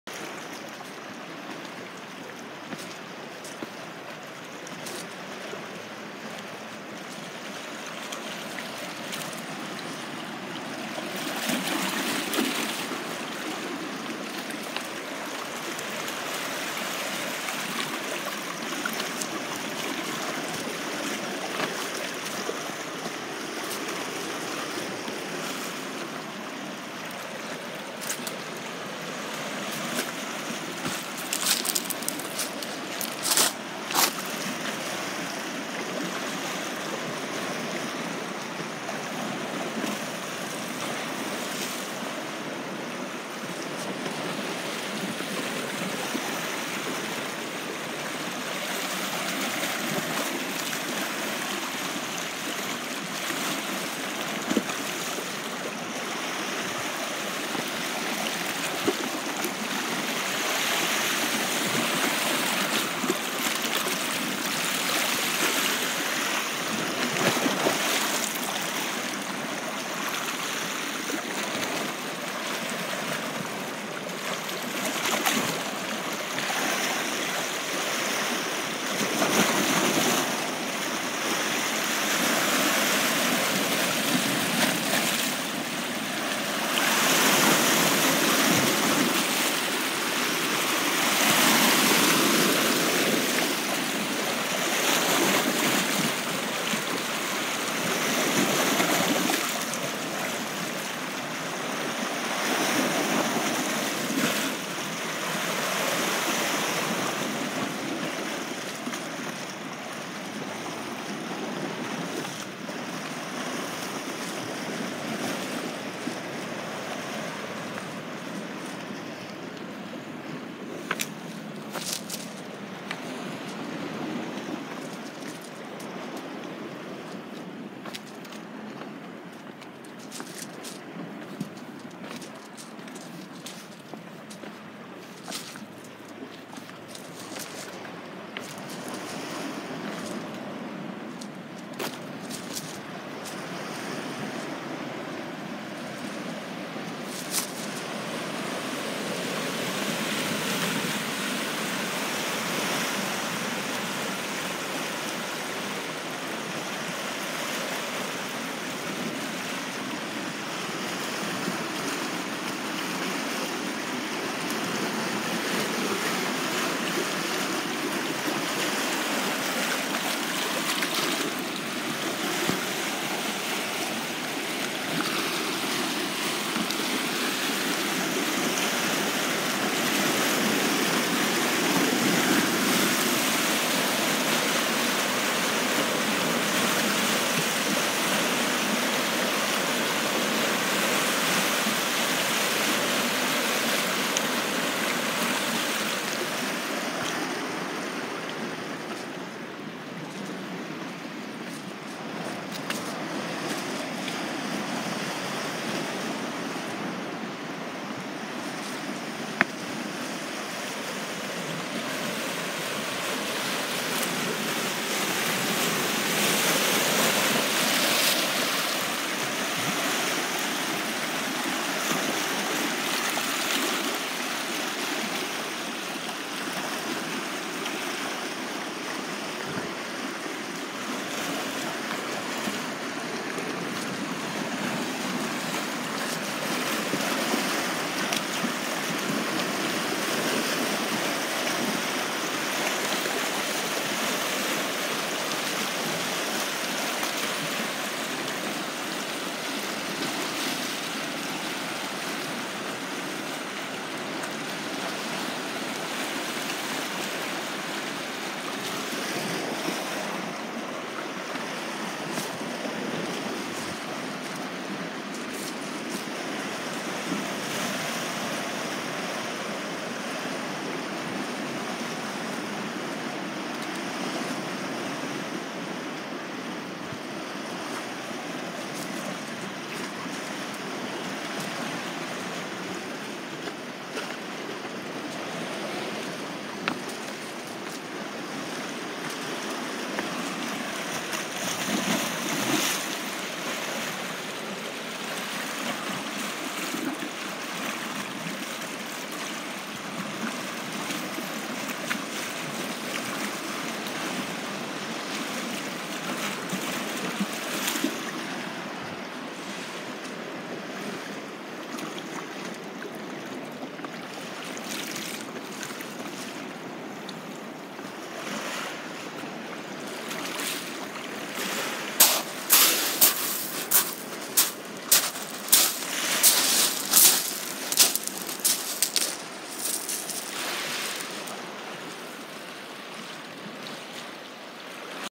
[ASMR] #1 Lake Superior Lava Pools..
An ankle twister of a hike, to say the least, the waves crash harder, making for a unique sound, enjoy this raw uncut journey.. *If wearing headphones, it’s unedited, so watch with lower volume first..